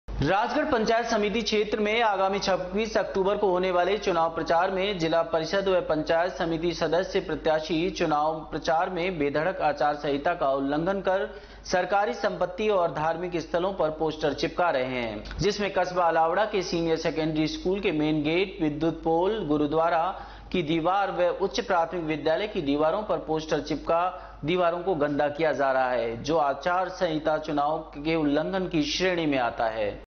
Alwarnews//प्रत्याशियों ने आचार संहिता का किया उल्लंघन// 22 10 2021
रामगढ पंचायत समीति क्षेत्र में आगामी 26अक्टूबर को होने वाले चुनाव प्रचार में जिला परिषद और पंचायत समीति सदस्य प्रत्याशी  चुनाव  प्रचार में बेधड़क आचार संहिता का उल्लंघन कर सरकारी सम्पति और धार्मिक स्थलों पर पोस्टर चिपका रहे हैं।जिसमें कस्बा अलावडा के सीनियर सैकेण्ड्री स्कूल के मैन गेट ,विद्युत पोल,गुरुद्वारा की दीवार  और उच्च प्राथमिक विद्यालय की दीवारों पर पोस्टर चिपका दीवारों को गंदा किया जा रहा है जो  आदर्श चुनाव आचार संहिता के उलंघन की श्रेणी में आता है।